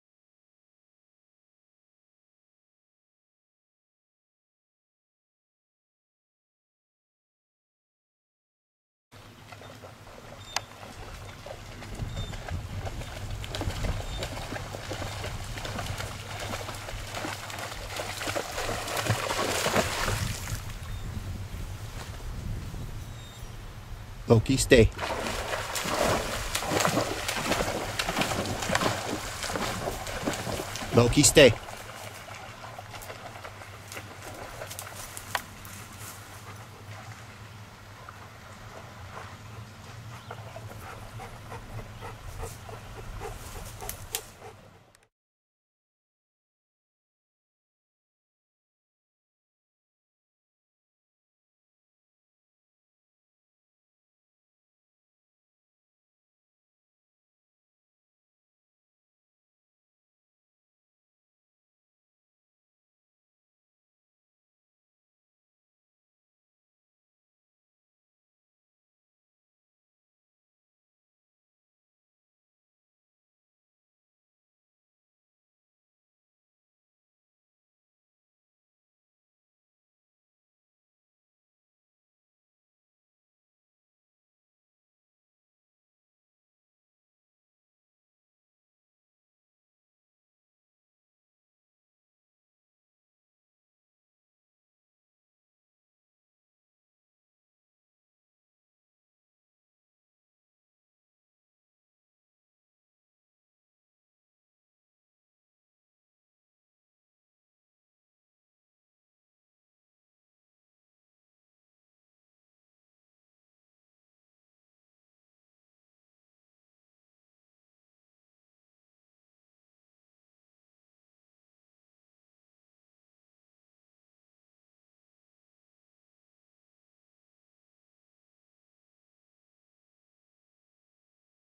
Buck in the Whitefish River
A short video of a young buck deer bounding down the Whitefish River...until he saw me.